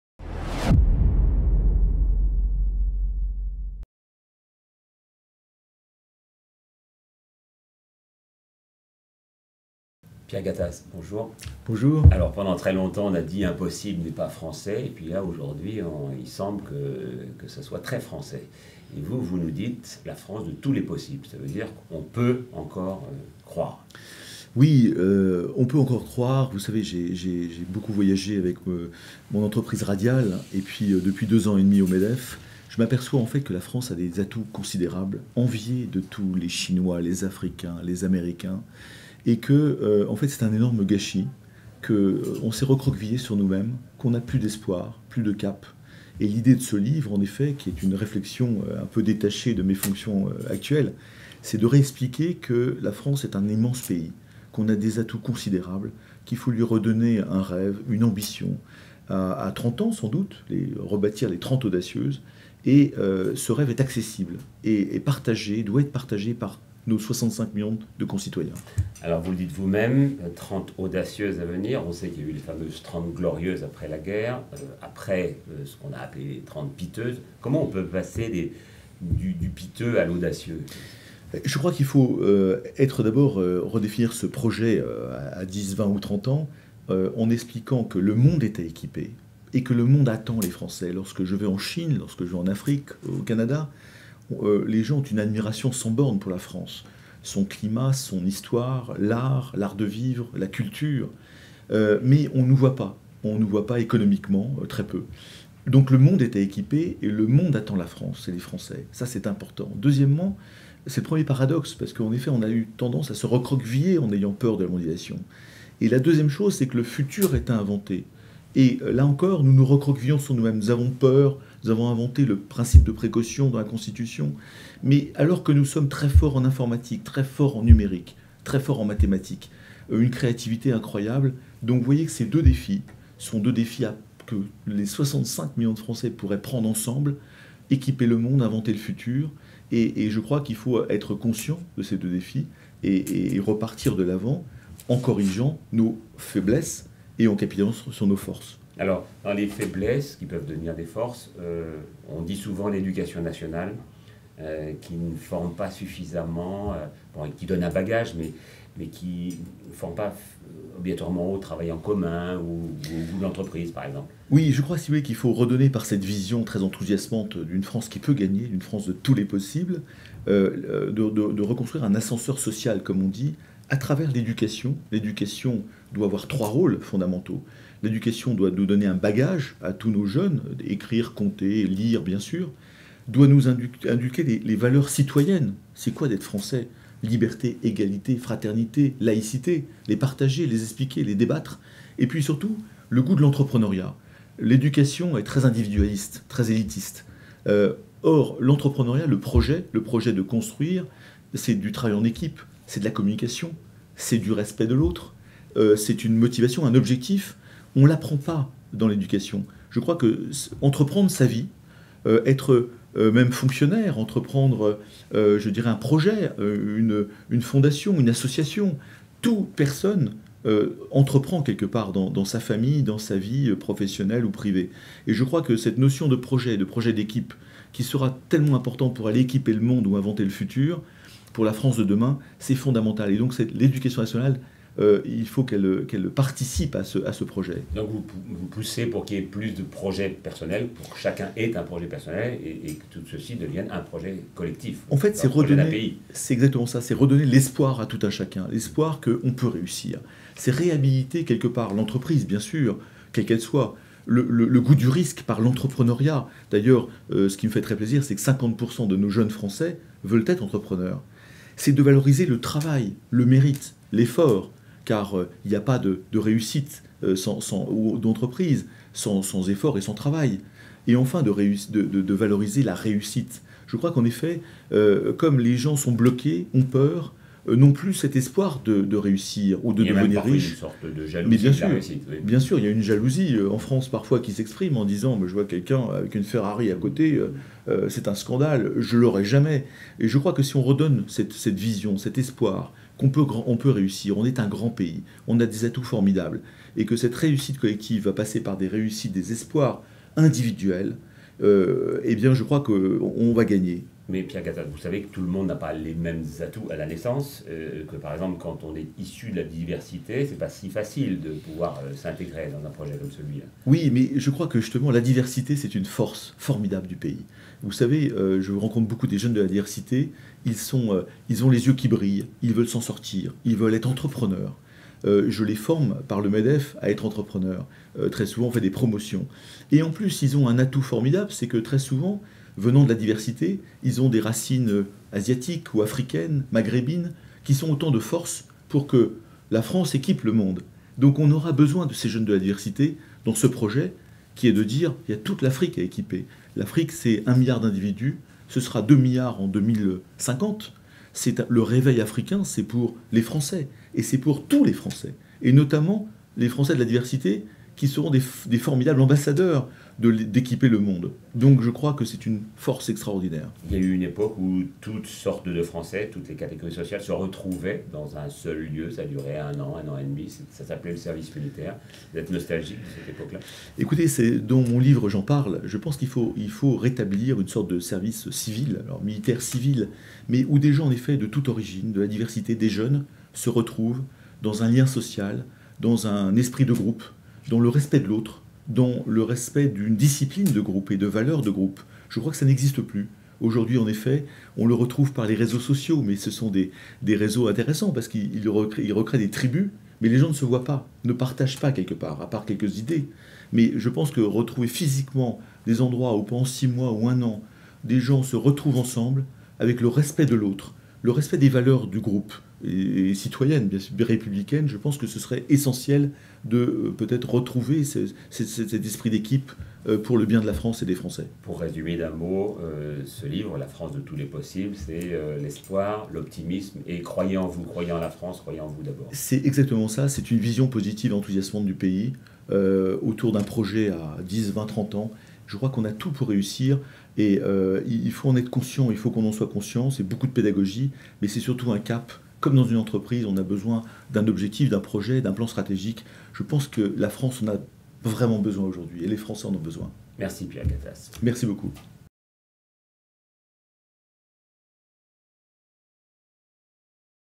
Questions à Pierre Gattaz par Patrick Poivre d'Arvor
L'auteur-dirigeant Pierre Gattaz, interviewé par Patrick Poivre d'Arvor, directeur de collection aux éditions Débats Publics, au sujet de son essai "La France de tous les possibles".